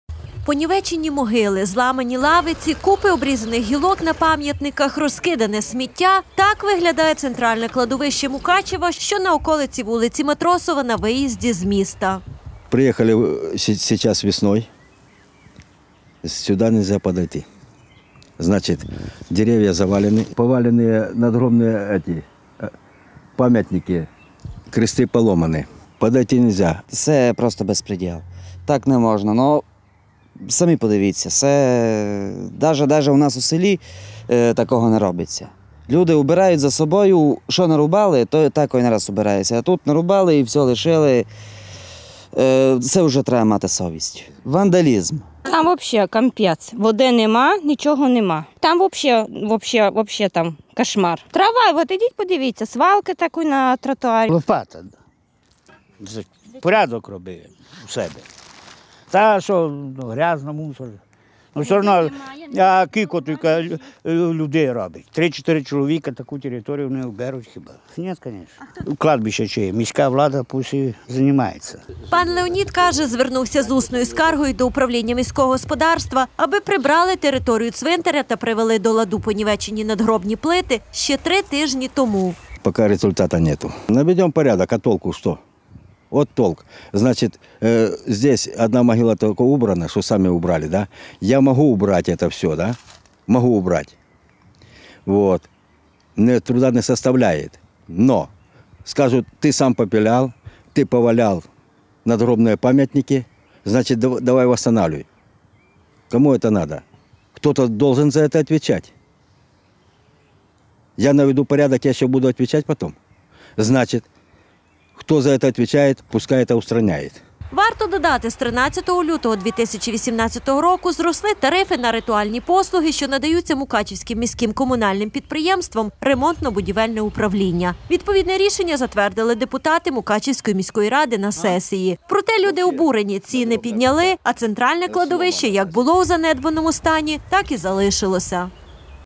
Центральне кладовище по вулиці Матросова на околиці Мукачева - у занедбаному стані
Люди обурені, адже мало того, що самотужки косять траву, прибирають сміття, доглядають за могилами , та ще й на території цвинтара - немає води – тому змушені носити з собою пляшки. Але точкою кипіння став інцидент після весняної обрізки дерев, коли комунальники не те що не прибрали гілки, а й понівечили пам’ятники та лавиці. Понівечені могили, зламані лавиці, купи обрізаних гілок на могилах, розкидане сміття - так виглядає центральне кладовище Мукачева, що на околиці вулиці Матросова на виїзді з міста. Варто додати, з 13 лютого 2018 року зросли тарифи на ритуальні послуги, що надаються Мукачівським міським комунальним підприємством «Ремонтно-будівельне управління». Відповідне рішення затвердили депутати Мукачівської міської ради на сесії. Проте люди обурені – ціни підняли , а центральне кладовище як було у занедбаному стані, так і залишилося.